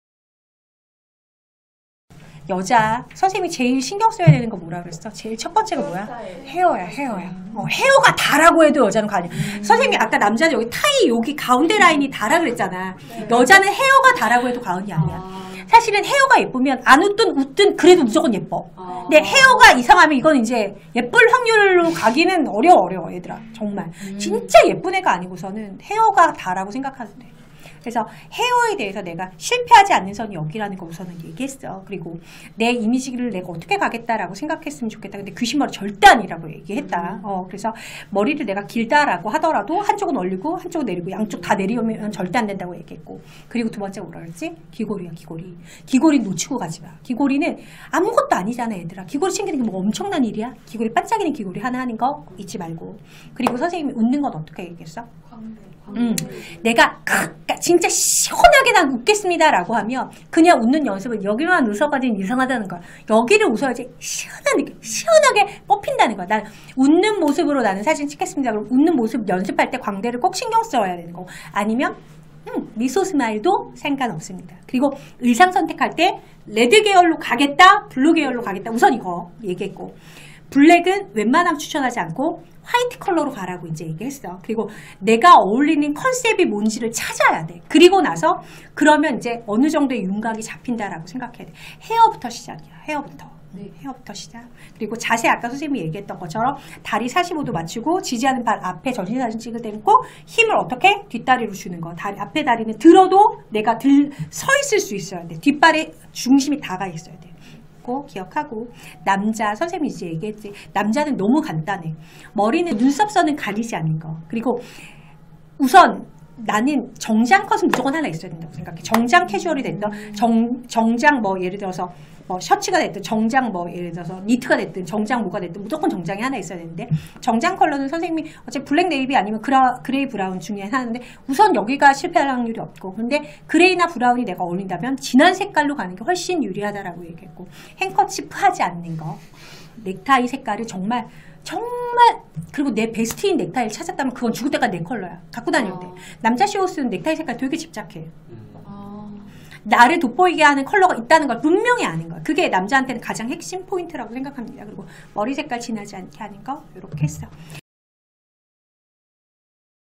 [김효석&송희영 쇼호스트 아카데미] 쇼호스트 공채 프로필사진 3분 정리
프로필 사진 찍는 방법 너무 어렵죠?
딱 3분!! 핵심내용만 확인하세요~